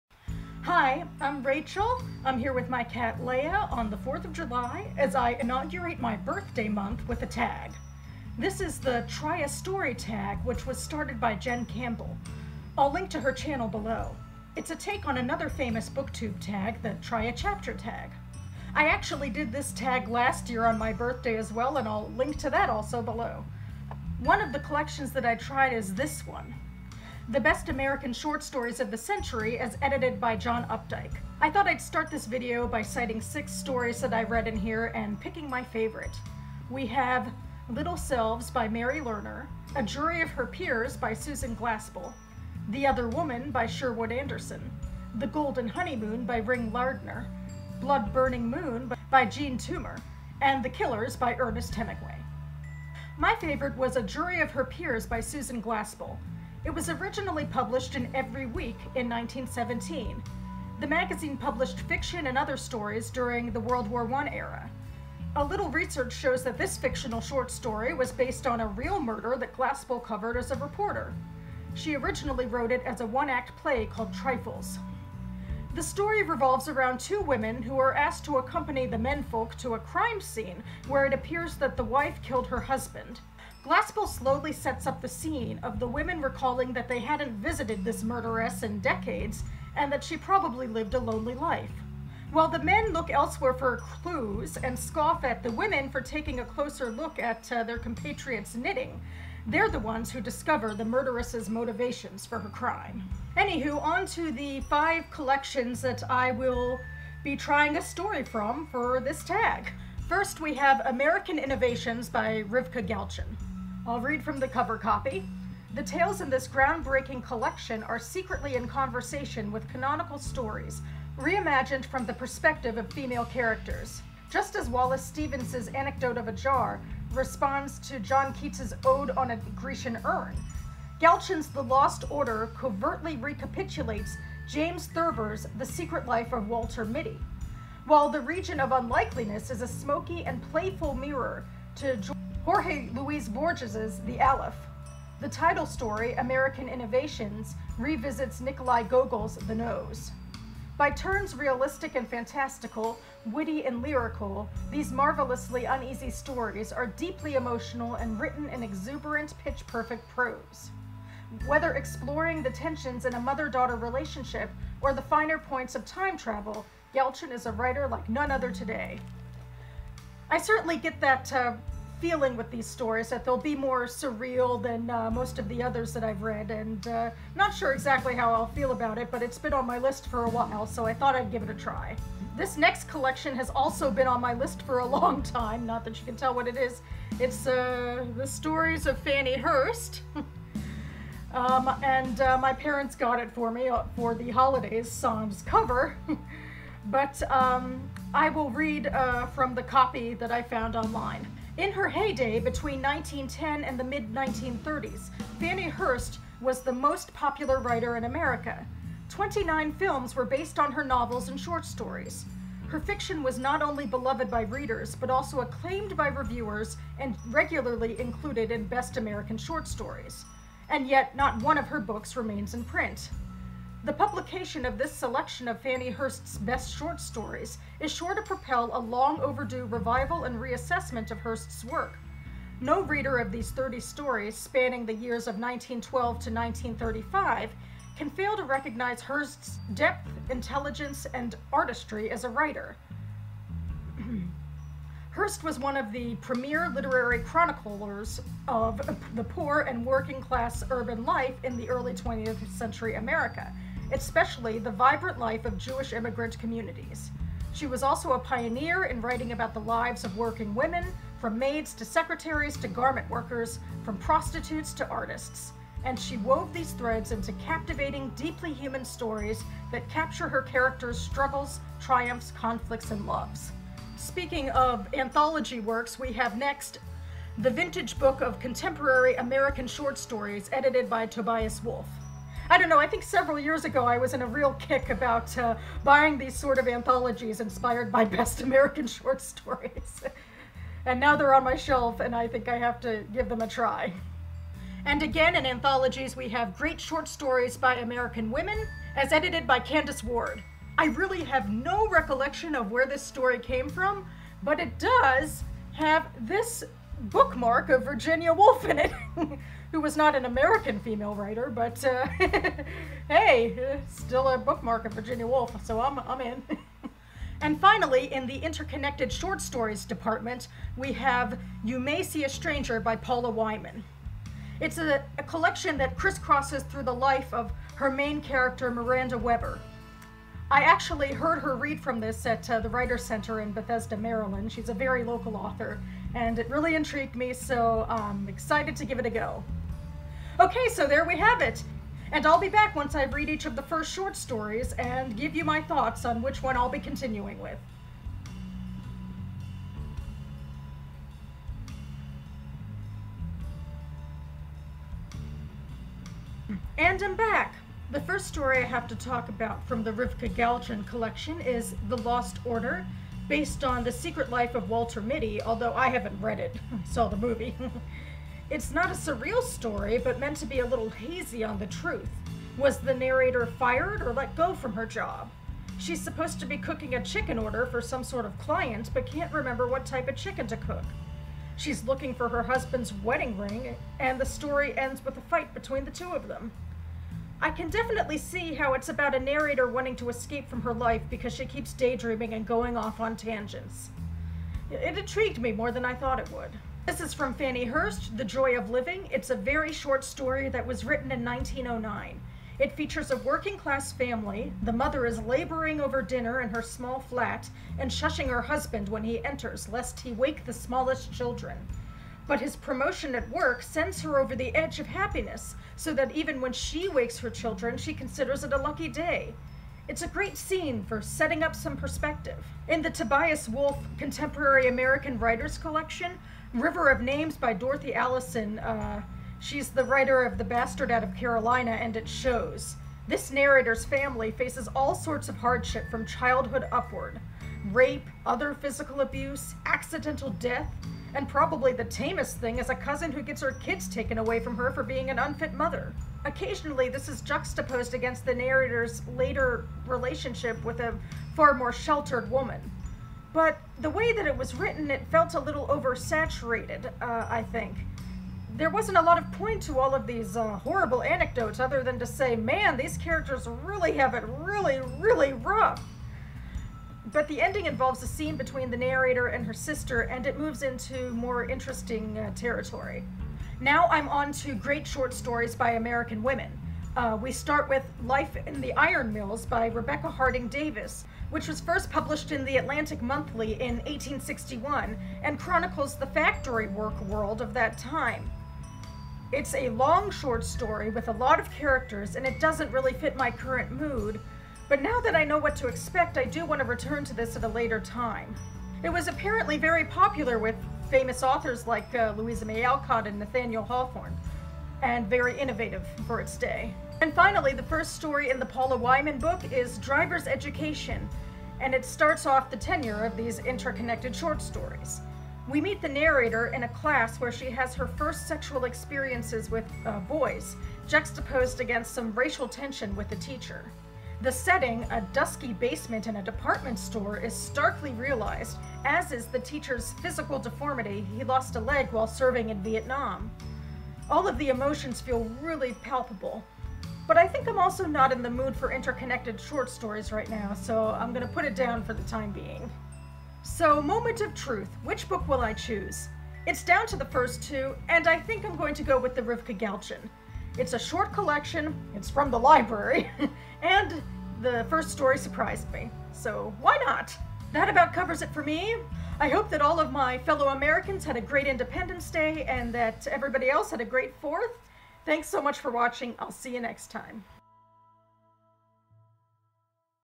2017 Try a Short Story Tag
In which I read and review the first short story from five collections.

THE VINTAGE BOOK OF CONTEMPORARY AMERICAN SHORT STORIES edited by Tobias Wolff